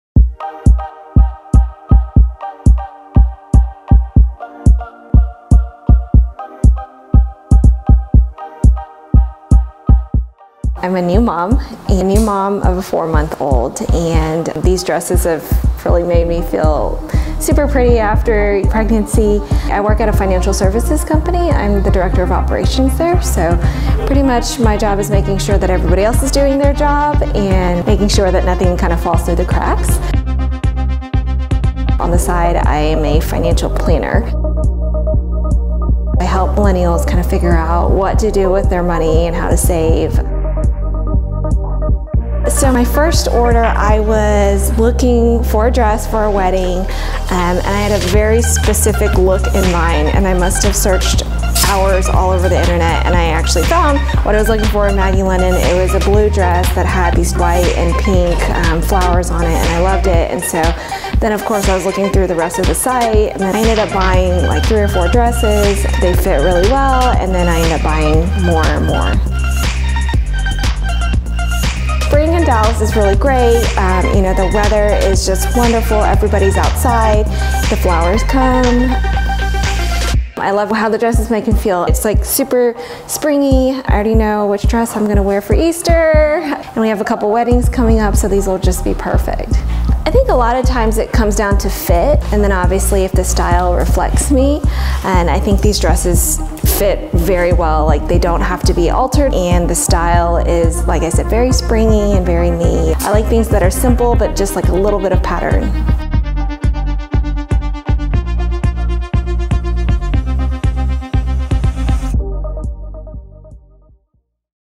Spring in Texas: Meet Emily
Meet Emily: a new mom balancing motherhood with her mission to empower young professionals to take charge of their money.